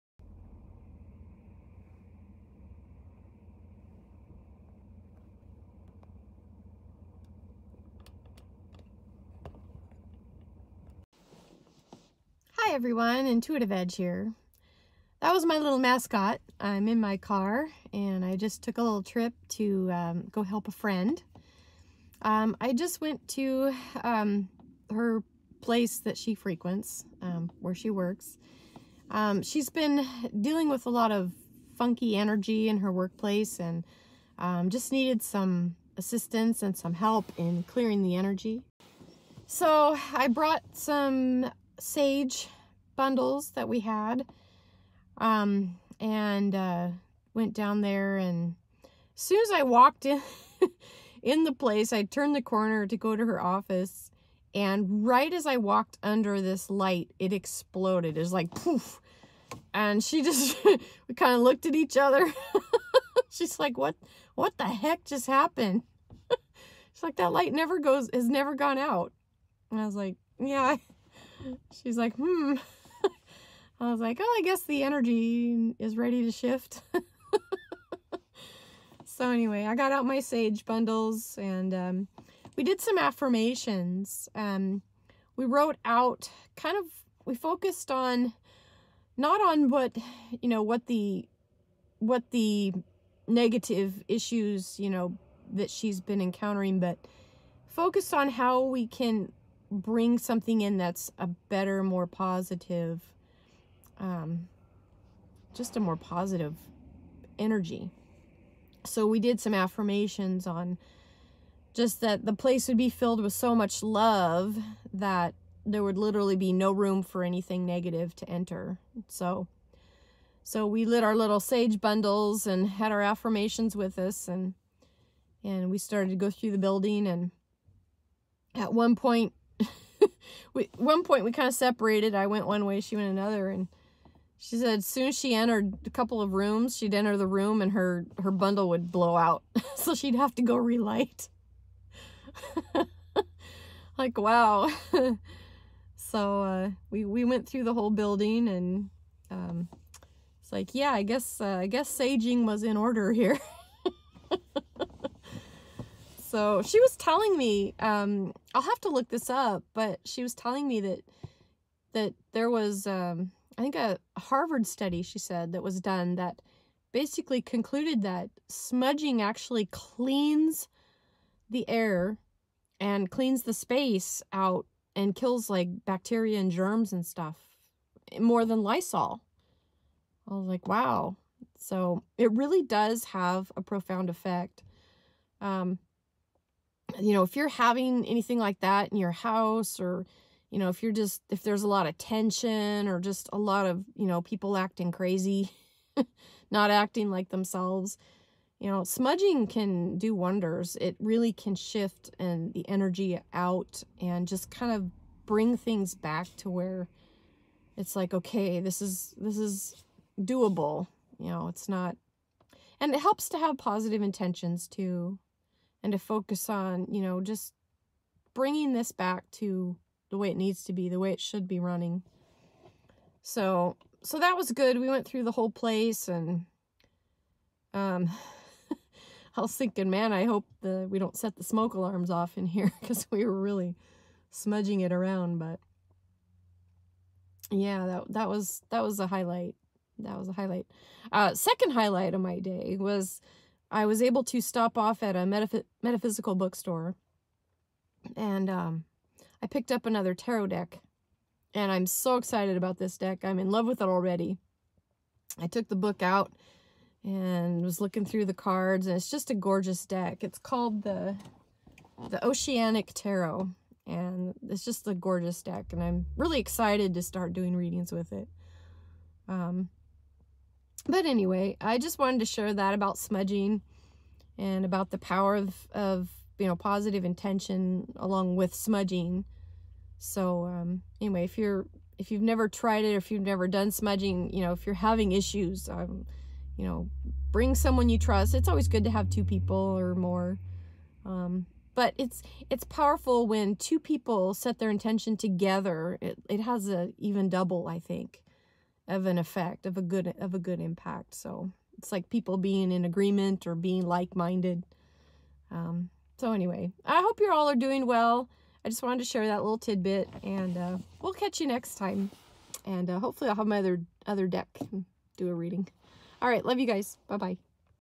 A Story About Smudging
In this video I relay an experience I had with smudging with sage and explain some of the important benefits to help clear space.